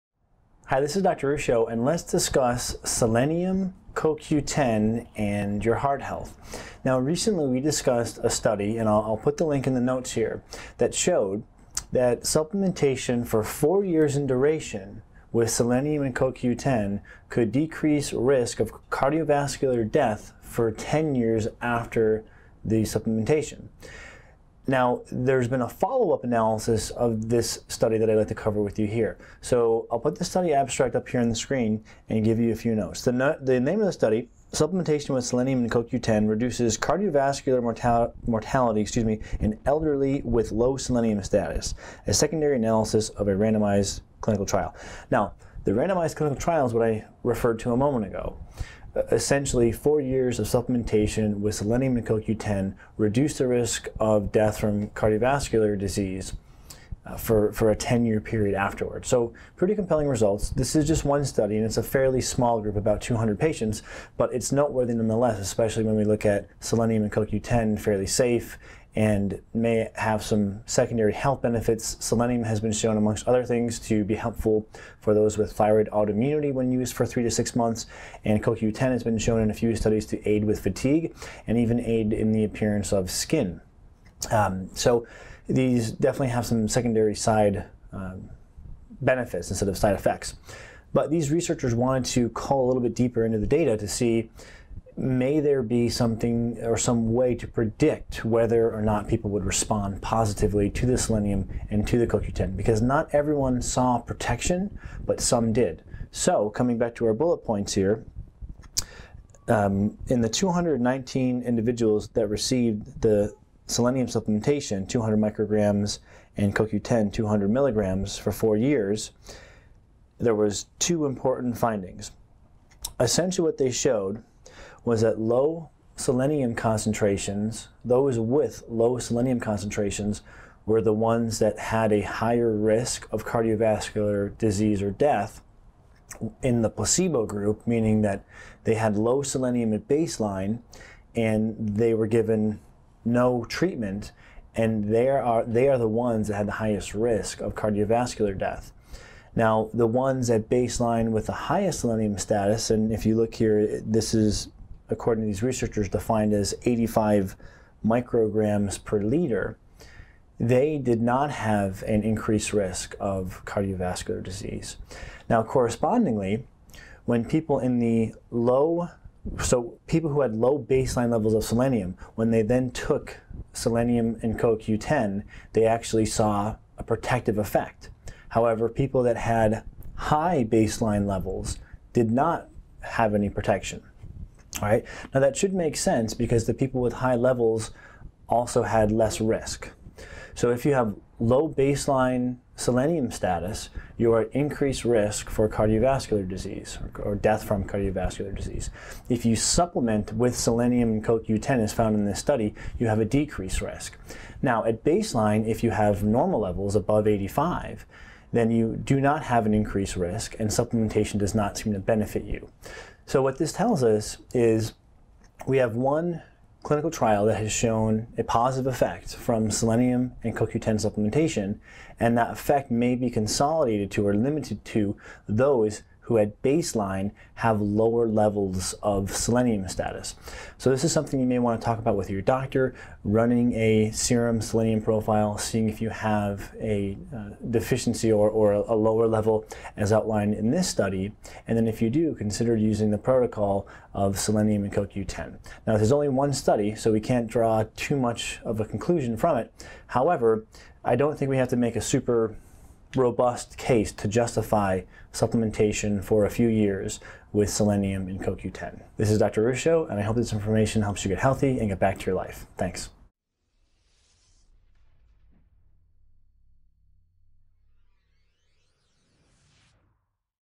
Selenium & CoQ10 Reduce Cardiovascular Death
We recently discussed a study in which selenium & CoQ10 supplementation reduced cardiovascular death.  Today, let’s discuss a follow up analysis that found that this protective effect was only seen in those with a baseline selenium deficiency and what values to look for on your lab work. Want more info?